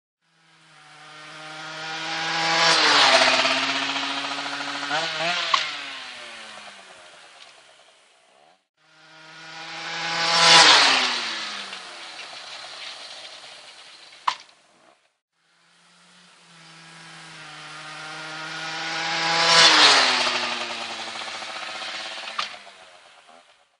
Rieju mrt stoppie
Moi qui débute les stoppie